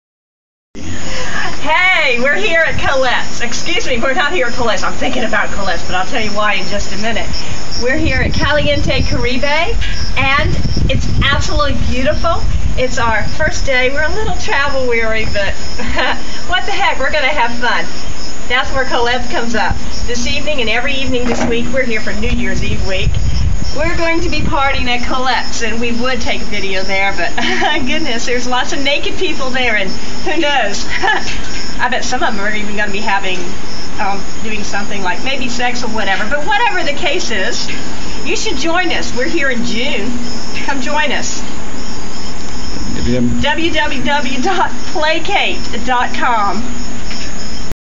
Going to Naked Party at Collettes Caribe in the Dominican Republic
New years week and Caribe in the DR